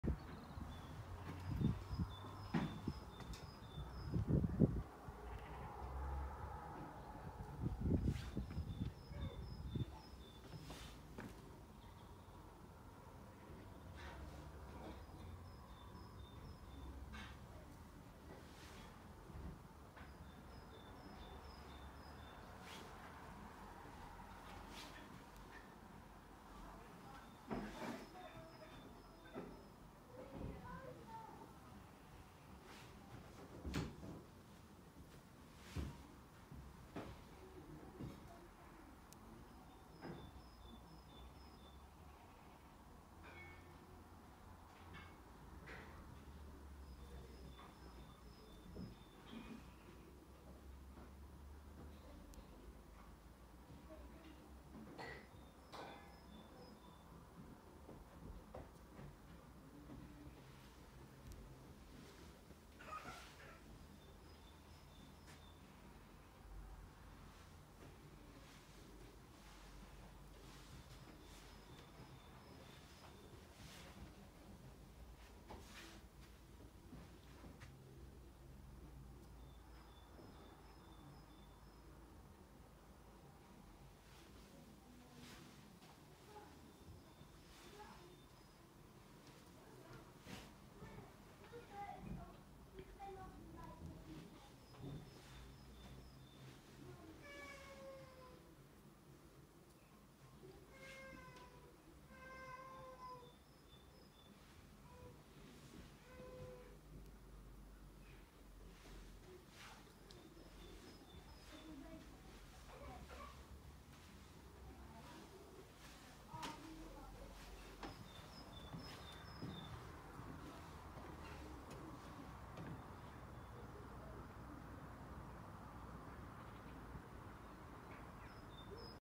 Jesters Day Nursery, Bicknacre- Baby room
Come with us to look round our baby room located in our small and rural day nursery in Bicknacre.